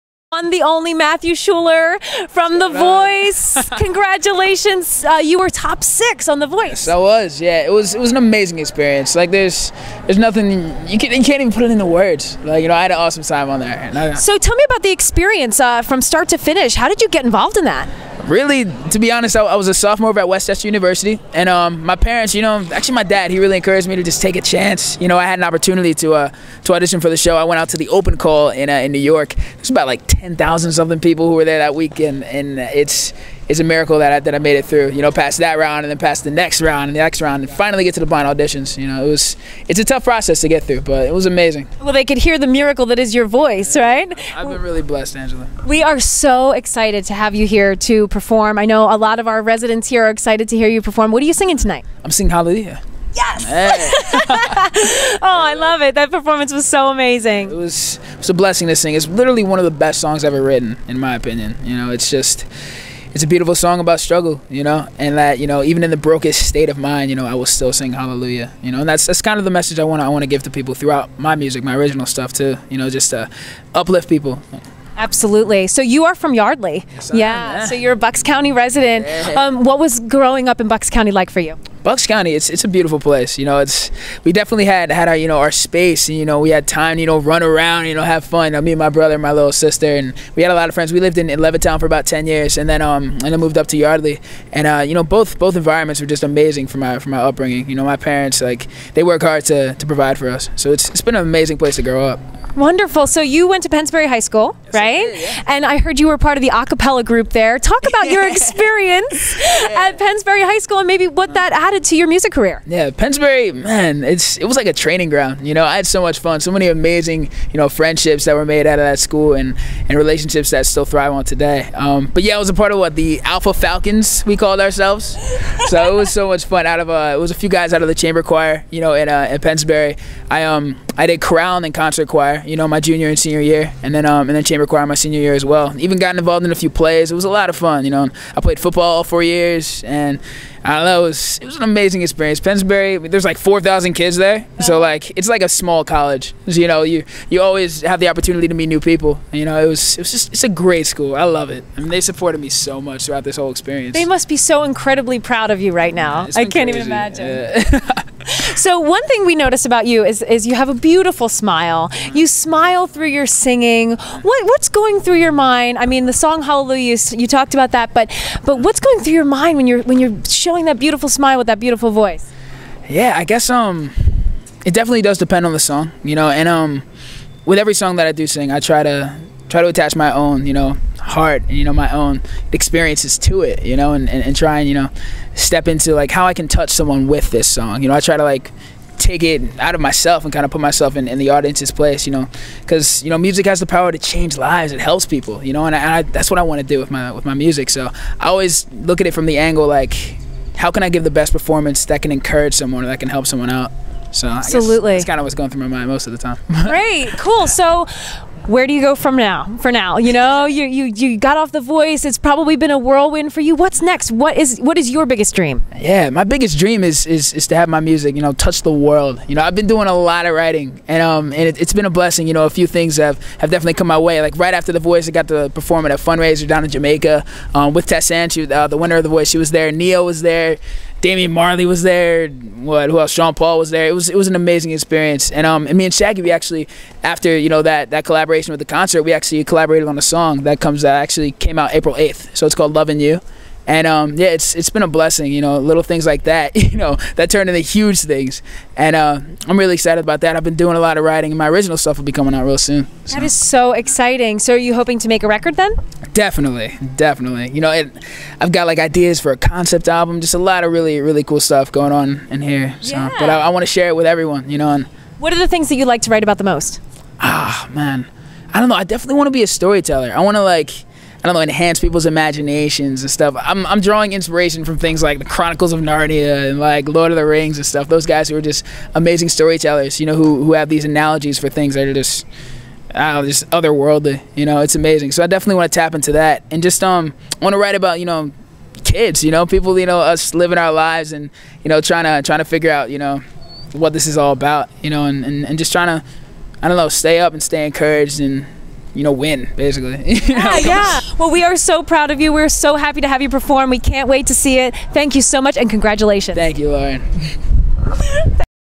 BH TV Interviews Matthew Schuler
Matthew Schuler gave a special performance at the 2014 Bucks Happening List Red Carpet Bash on April 10, 2014 at Pen Ryn Estate. BH TV's Lauren Rooney caught up with the rising star from Yardley